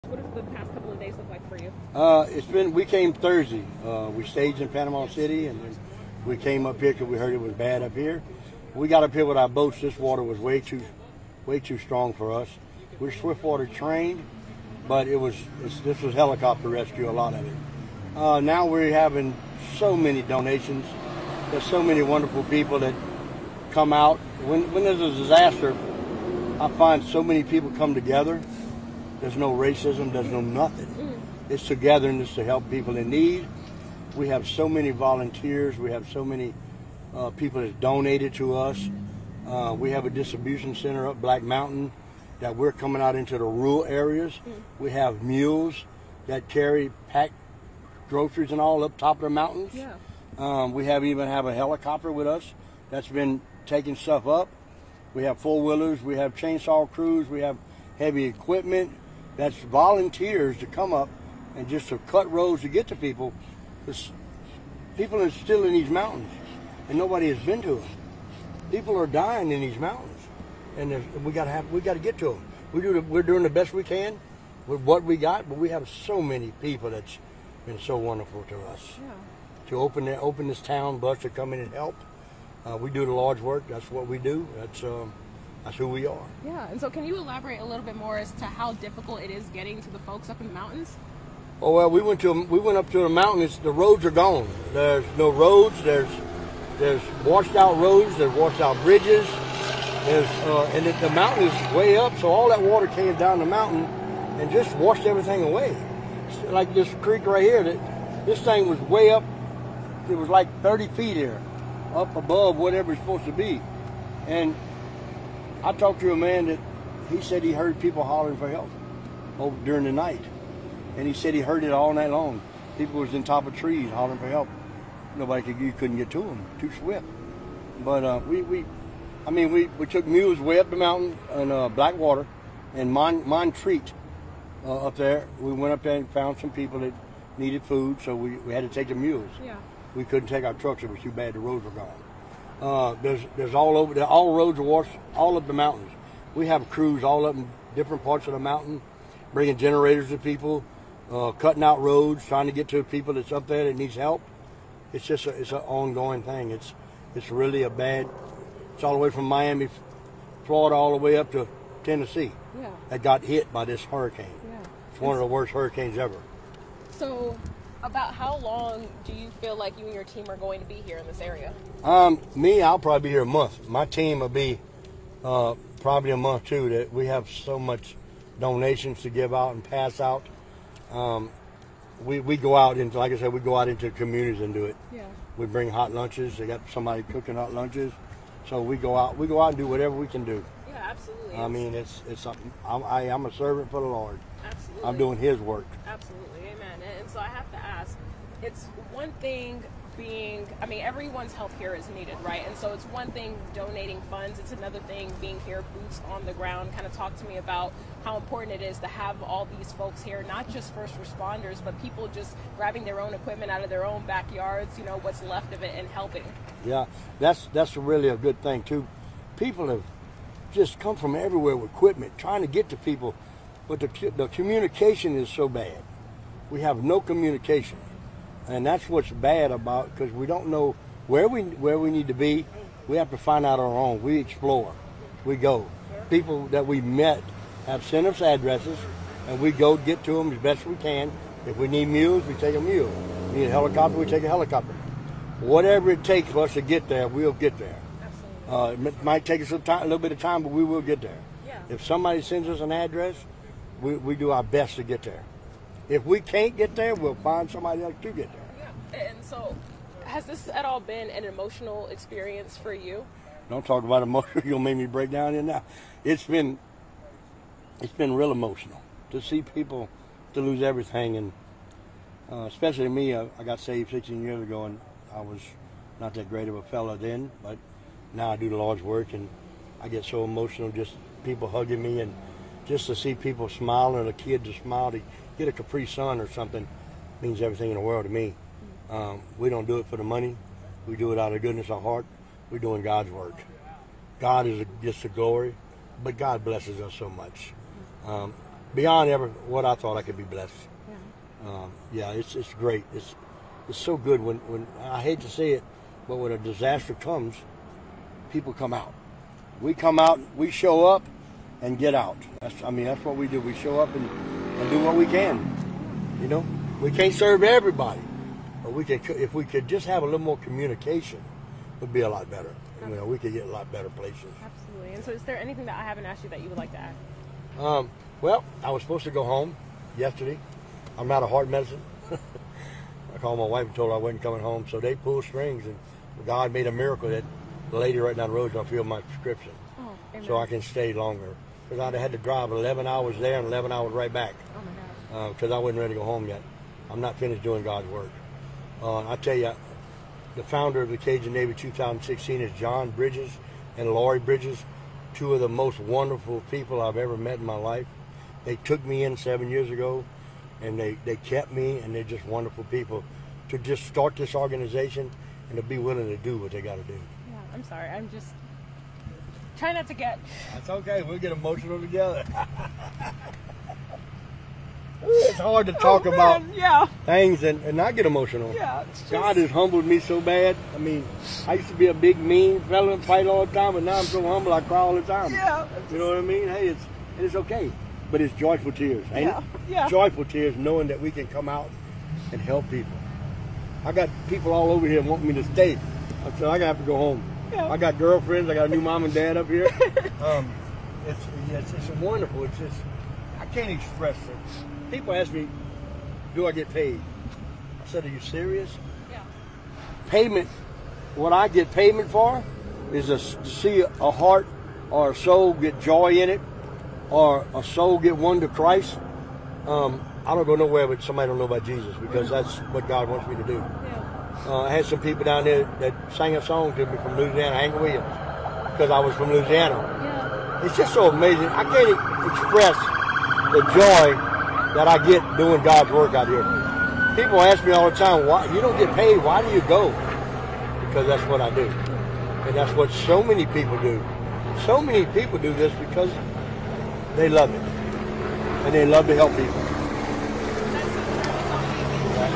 NBC 10 speaks with residents in North Carolina amid Hurricane Helene
NBC 10's Temi-Tope Adeleye spoke with residents after the devastating damage in North Carolina.